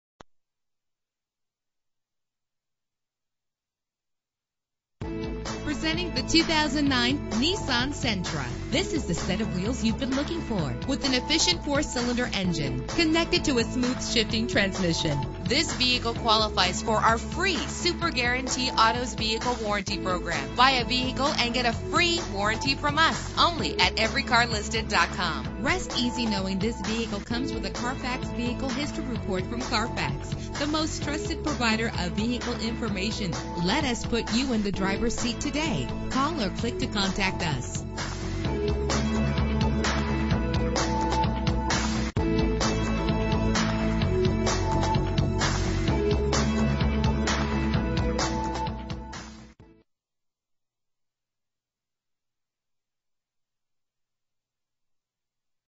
2009 Nissan Sentra - Richardson TX
Year: 2009
Make: Nissan
Model: Sentra
Trim: 2.0 FE+
Engine: 2.0 liter inline 4 cylinder 16 valve
Transmission: Continuously Variable
Color: Red
Mileage: 32628
Address:
1777 North Central Expressway